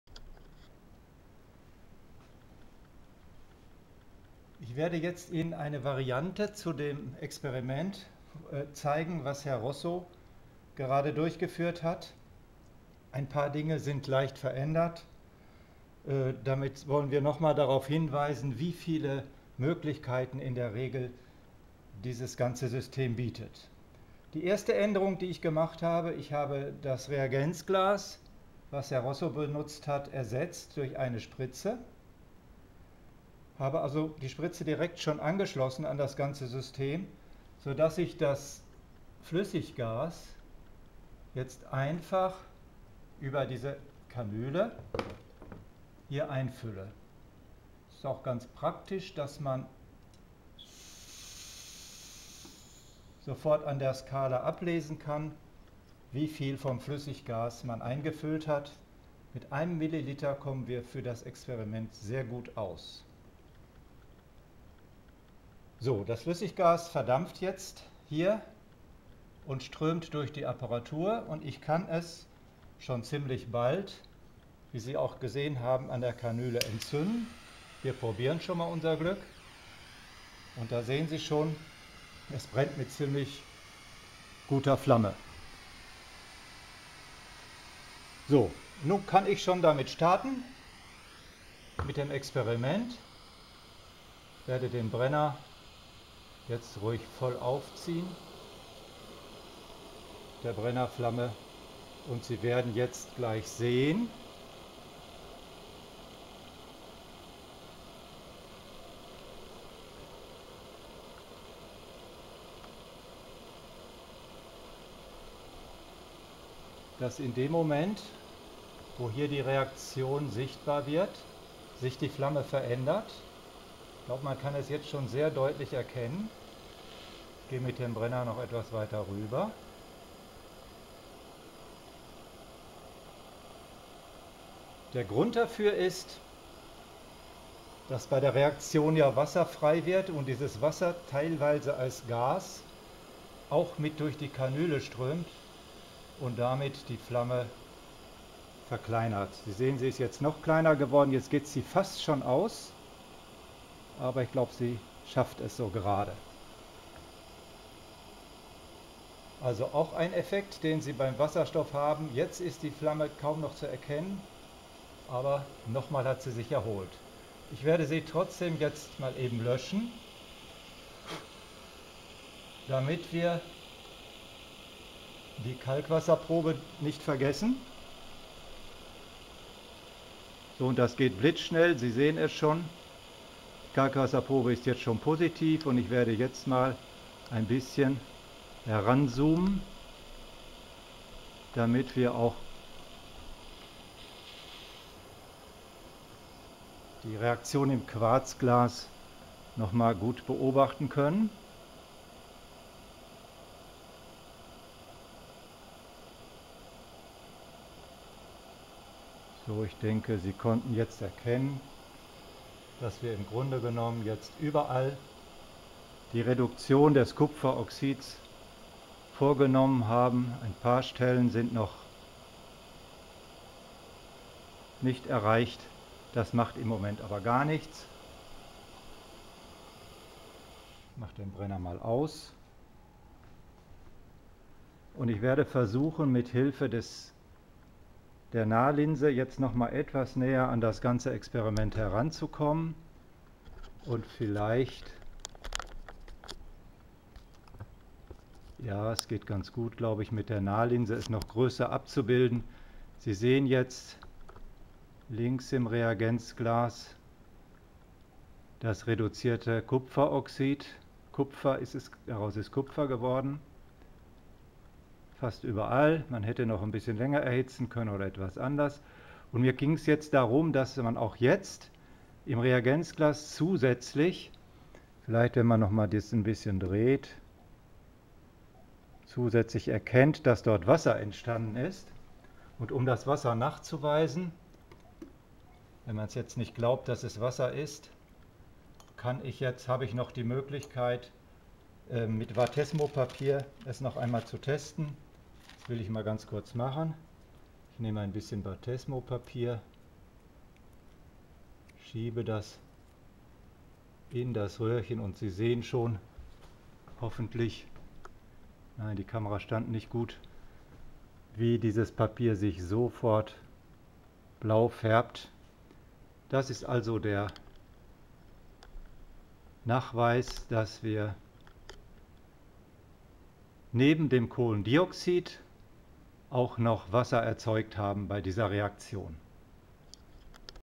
Reduktion von KupferIIoxid 3
Kupferoxid wird mit Flüssiggas reduziert (Variante nach Grofe)